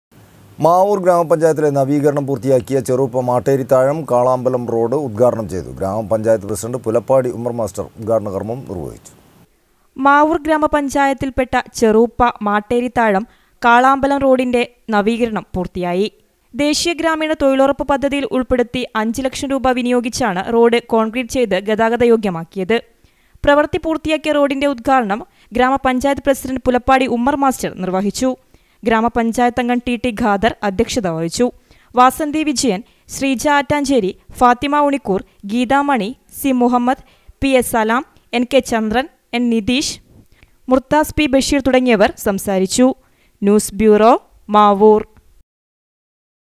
മാവൂർ ഗ്രാമപഞ്ചായത്തിലെ നവീകരണം പൂർത്തിയാക്കിയ ചെറൂപ്പ മാട്ടേരിതാഴം - കാളാമ്പലം റോഡ് ഉദ്ഘാടനം ചെയ്തു
മാവൂർ ഗ്രാമപഞ്ചായത്തിലെ നവീകരണം പൂർത്തിയാക്കിയ ചെറൂപ്പ മാട്ടേരിതാഴം - കാളാമ്പലം റോഡ് ഉദ്ഘാടനം ചെയ്തു. ഗ്രാമ പഞ്ചായത്ത് പ്രസി: പുലപ്പാടി ഉമ്മർ മാസ്റ്റർ ഉദ്ഘാടന കർമം നിർവഹിച്ചു